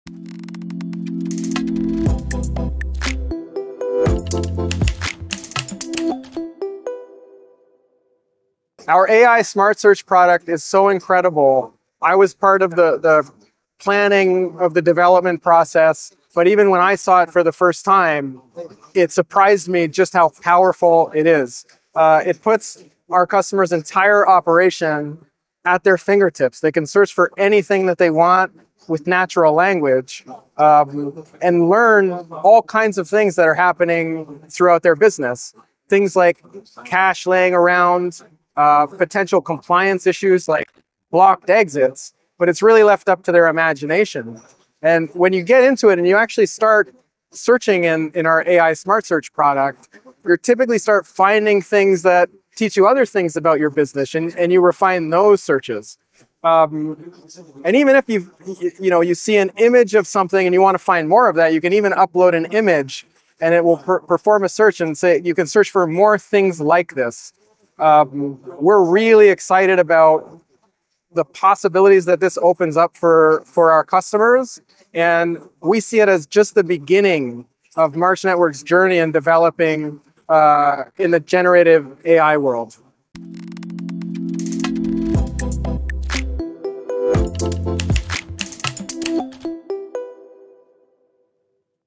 Introduction to March Networks' AI Smart Search
Learn more about the latest innovation from March Networks, their AI Smart Search solution as demonstrated during Intersec Dubai 2025